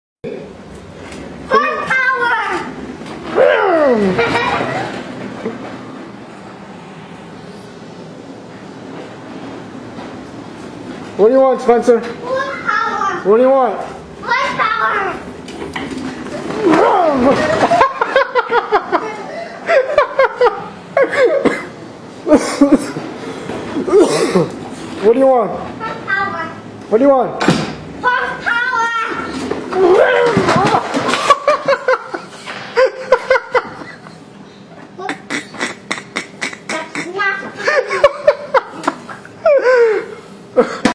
Man Pushes Back Of Son's Tricycle Too Hard, Boy Falls Over
A boy rides a tricycle around the floor at home and asks for horsepower from his father, who kicks the back of the tricycle and pushes him down the hallway. On the third lap the man pushes too hard and knocks the boy over.